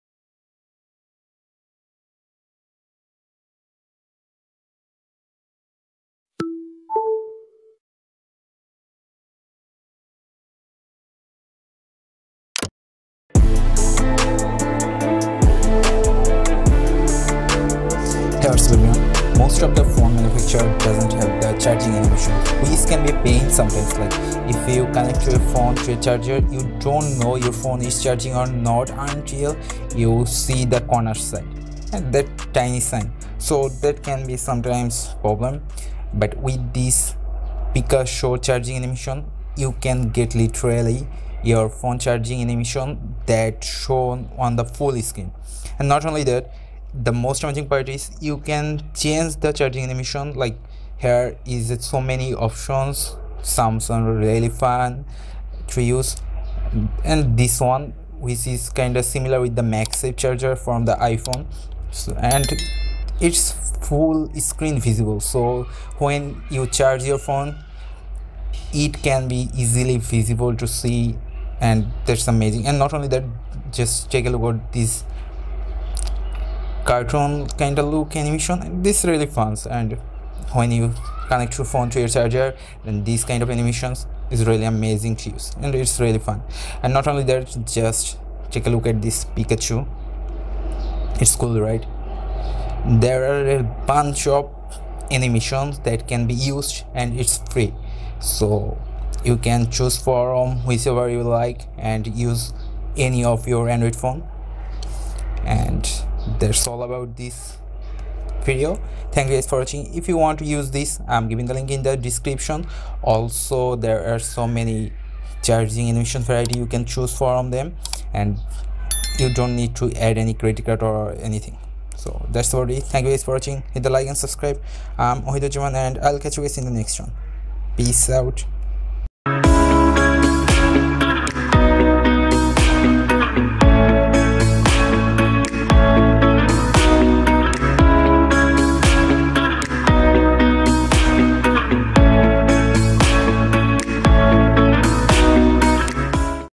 Get Charging Animation/Effect on Any Android Phone without root ft. MagSafe Android, Squid Game, etc
Many phone manufacturers don't have charging connected animation or some do have but that doesn't look like a good one.
So now, with this app you can get a variety of charging animation/effect and you will love this. It has iPhone Magsafe animation (my preference), squid game effect, anime, pikachu, and so many more you cant imagine and these all are free with a simple ad only.
It also has battery widgets.

Download and Install Charging Animation/Effect on Any Android Phone without root

🔥More stuff you may like -